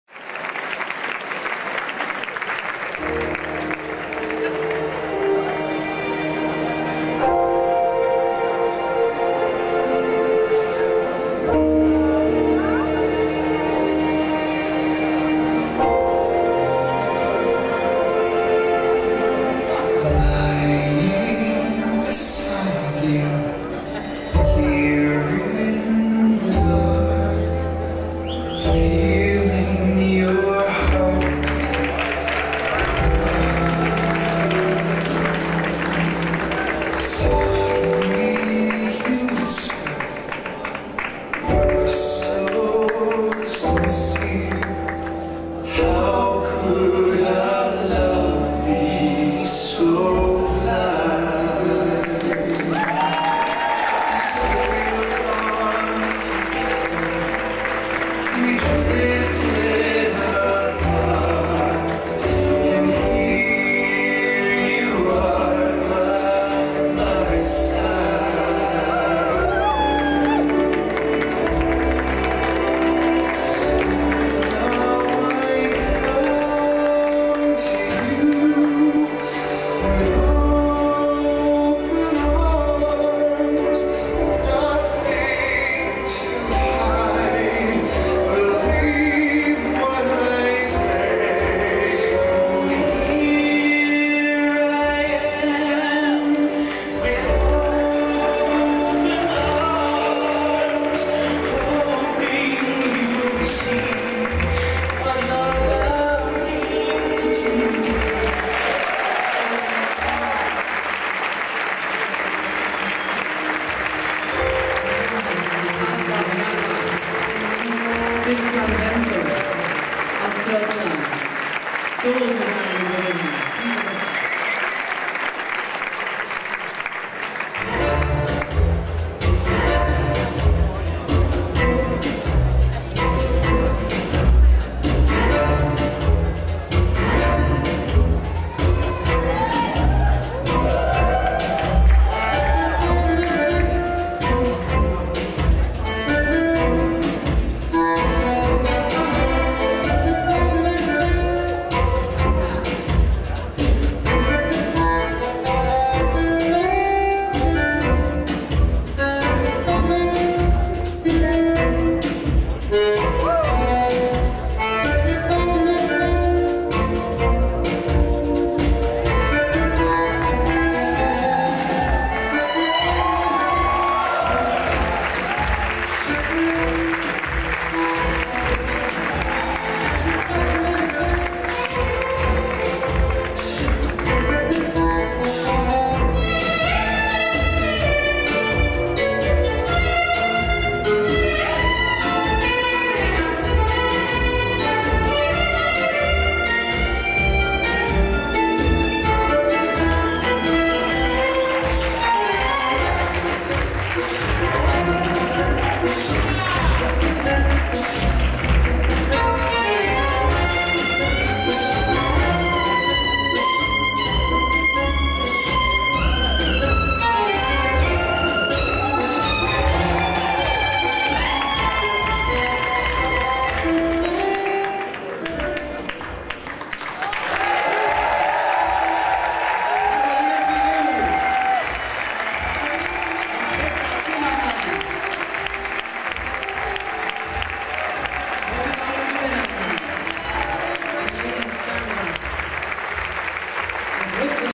Slow Walz & Tango of the Gold Medal Winners Standard
Show der Goldmedalliengewinner in den Standardtänzen bei den Gay Games Colgne 2010 am Abend des Unity Balls